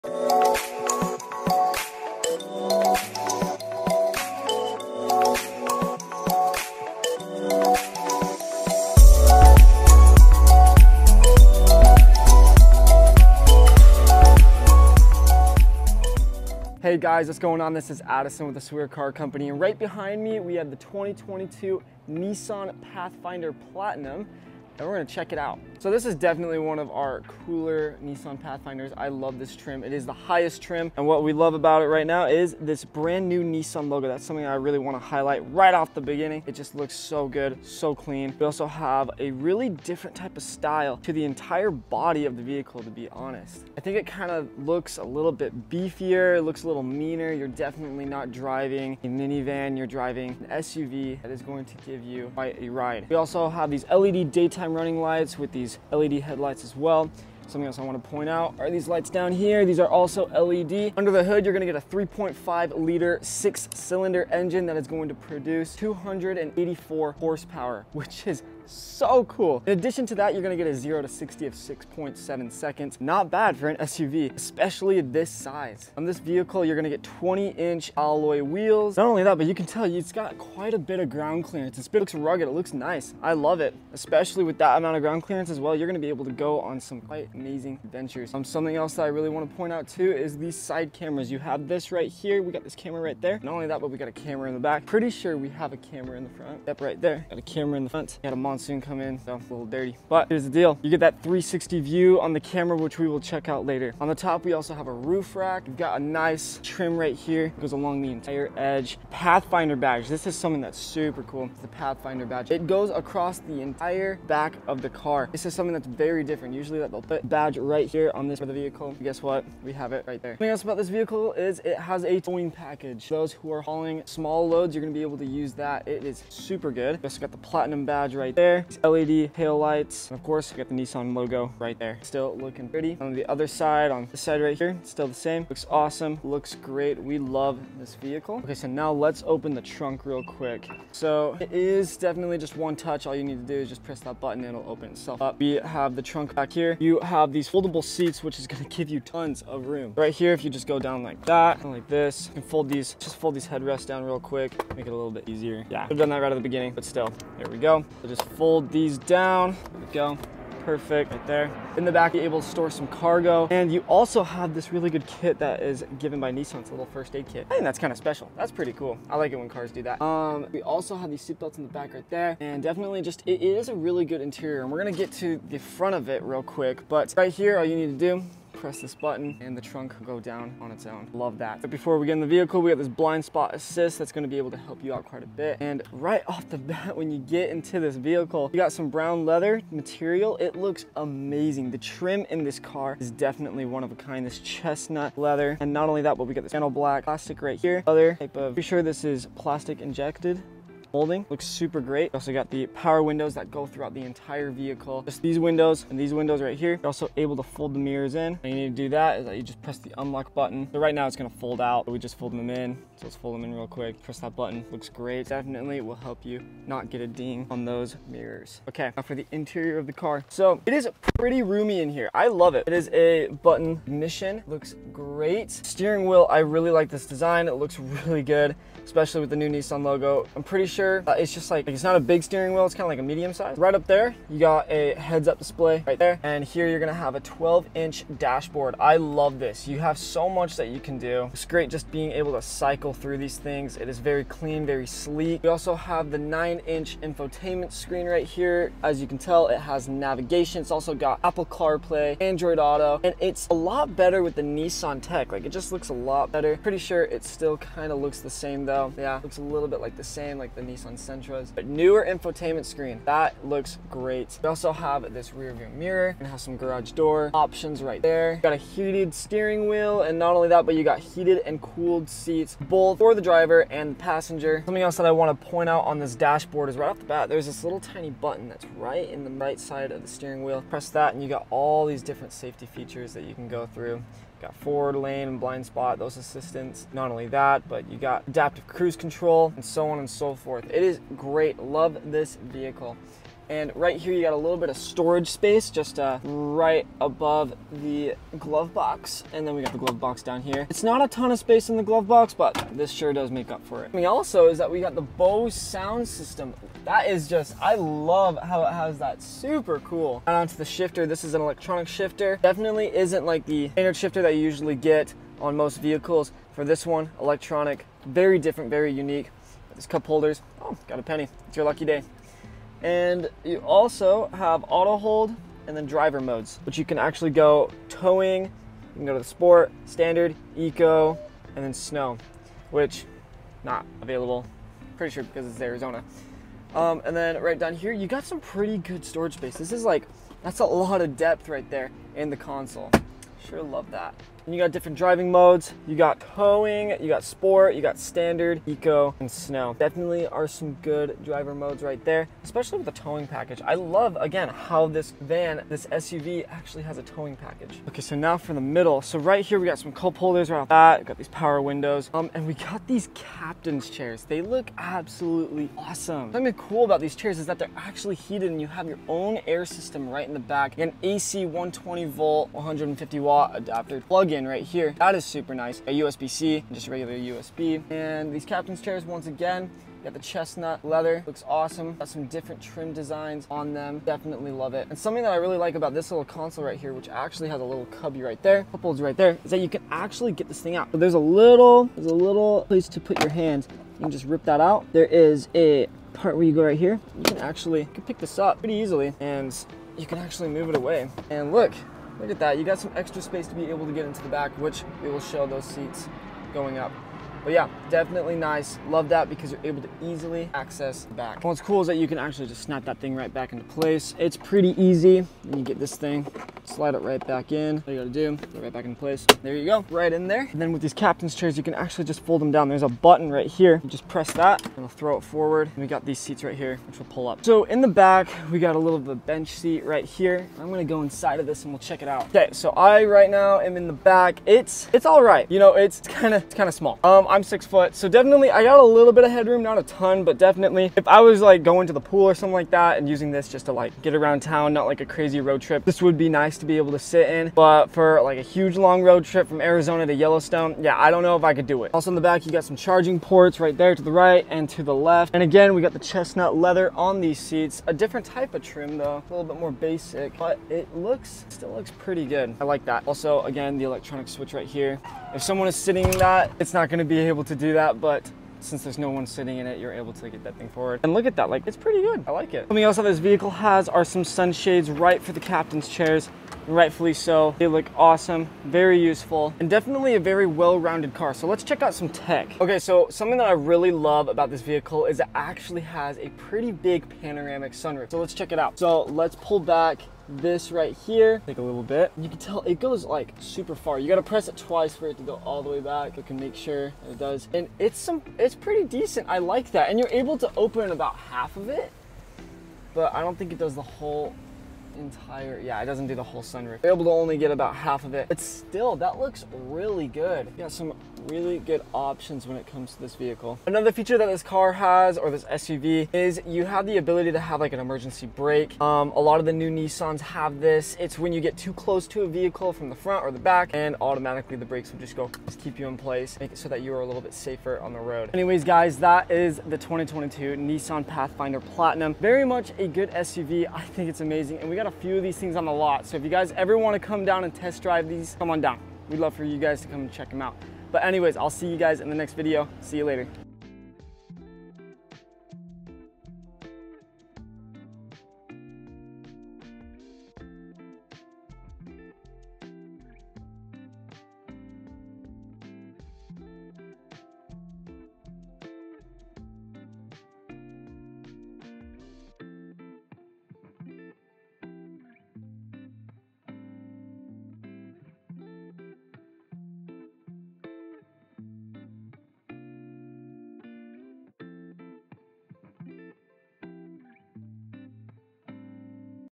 2022 Nissan Pathfinder Platinum: FULL WALKTHROUGH
Click the link below to find your next used vehicle with the best car crew in the business! We want to help you find the best car for your needs!